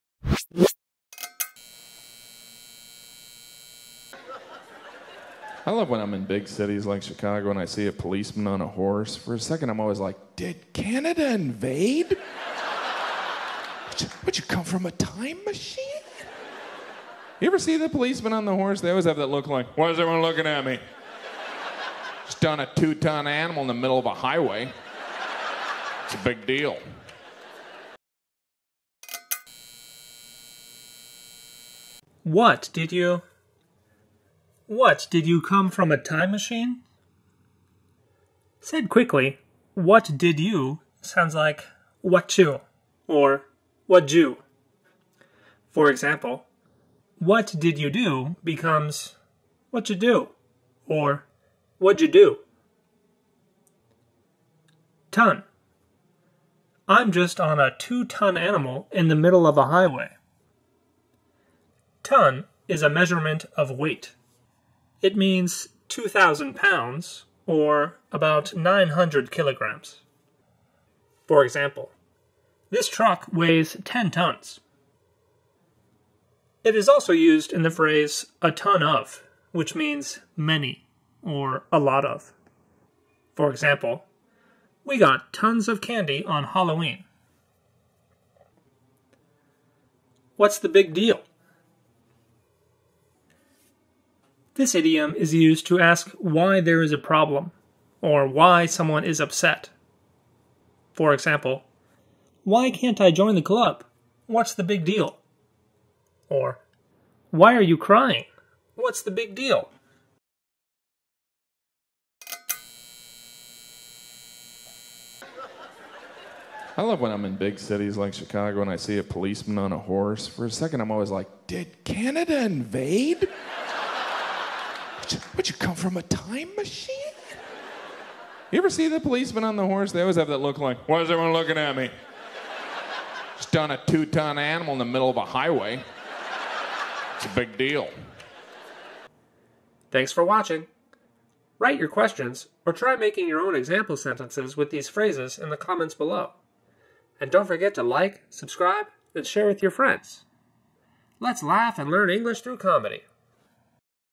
Laugh and Learn English: Jim Gaffigan - Horseback Police
This video's vocabulary:
Why did you... [elision]
ton [noun]
What's the big deal? [idiom]